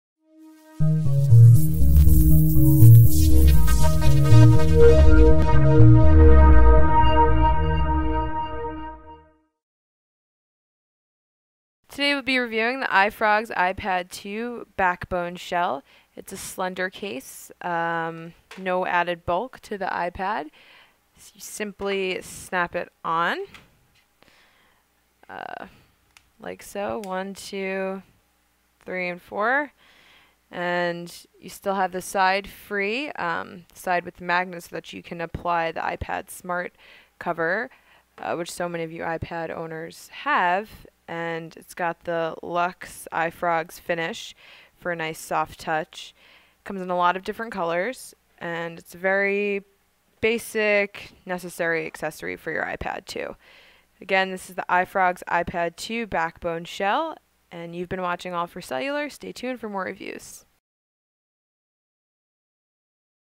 iFrogz iPad 2 Backbone Shell Video Review
Comprised of high-quality polycarbonate materials, the slender iFrogz iPad 2 Backbone Shell will shield your Apple iPad 2 from dents and scratches without adding unnecessary bulk. The Backbone Shell is compatible with the Apple Smart Cover so your iPad 2 gets the protection it needs in the front and back. The iFrogz iPad 2 Backbone Shell features a Luxe finish to give your case a soft-touch feel. Made with accessibility in mind, the Backbone Shell allows access to all ports and buttons on your iPad 2.